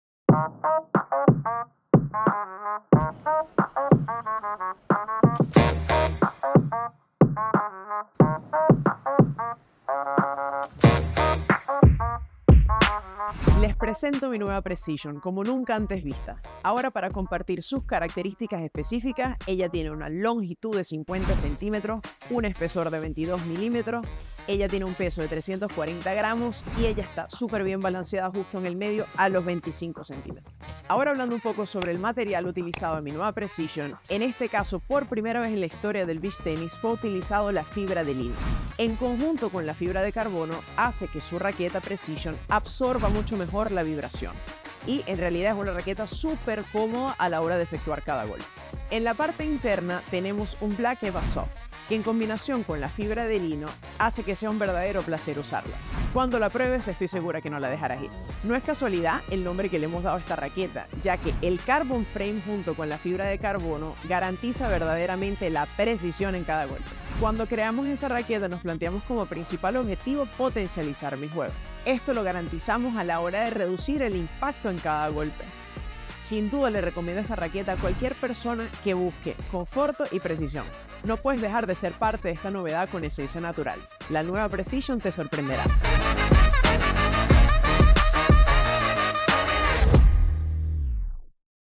Behind the racket - new Sandever PRECISION BTPRO racket by Patty Diaz [SUB. PT - ENG - ITA]
Patty Diaz, Beach tennis world champion, tells us the features and secrets of her new Sandever PRECISION BTR PRO racket.